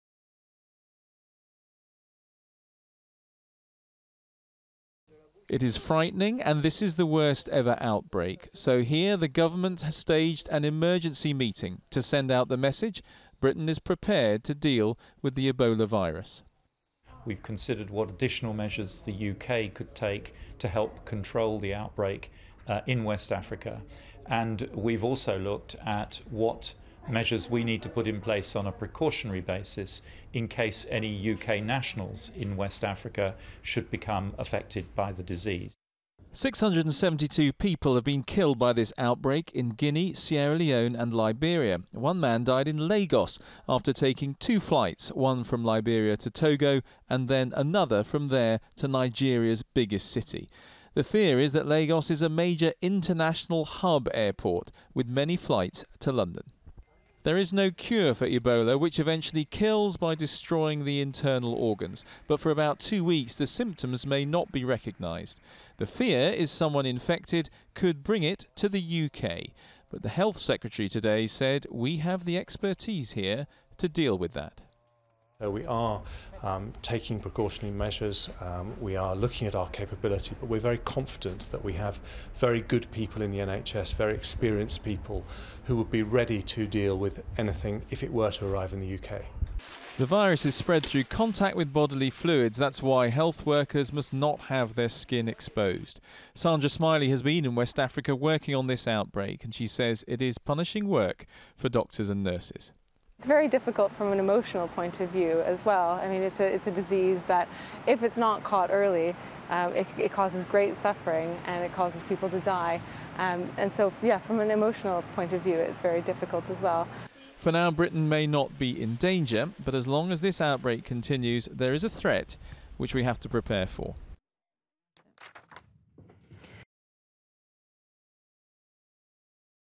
Government holds emergency meeting as fears grow of Ebola outbreak in Britain
Doctors have been told to look out for symptoms of the disease which has already killed hundreds.